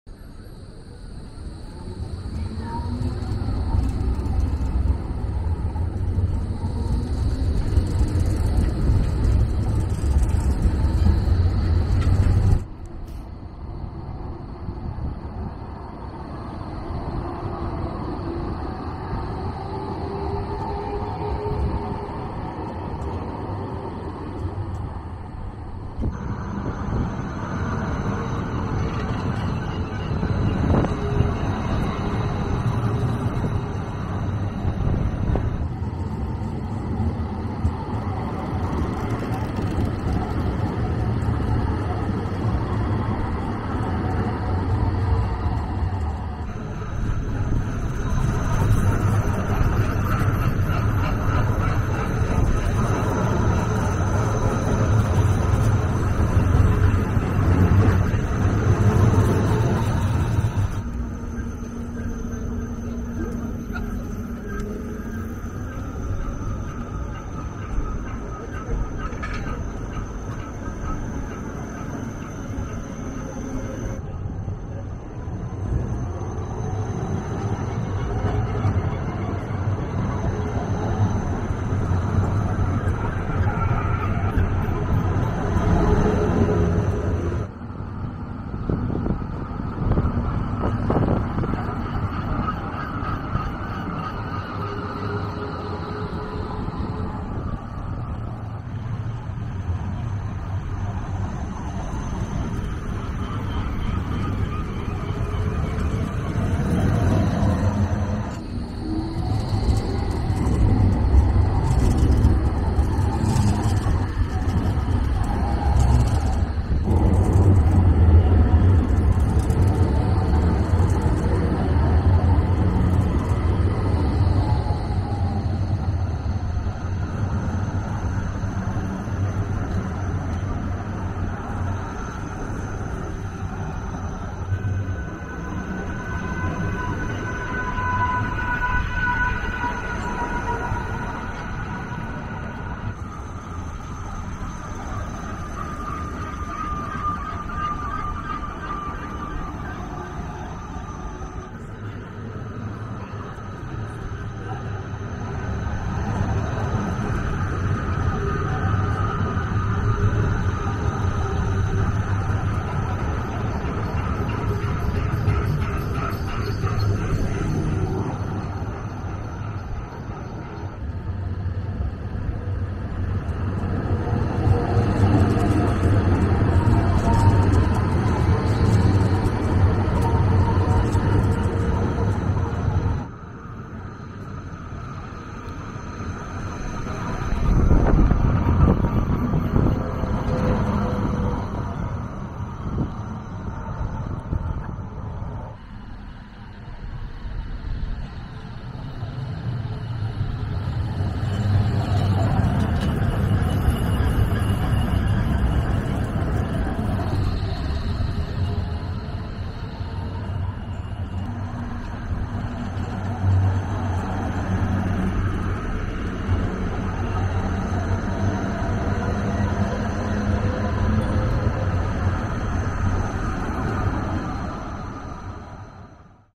Straßenbahnen Halle Saale 7 2023 4K
Straßenbahnen in Halle Saale Anfang Juli 2023 im Berufsverkehr und natürlich in 4K.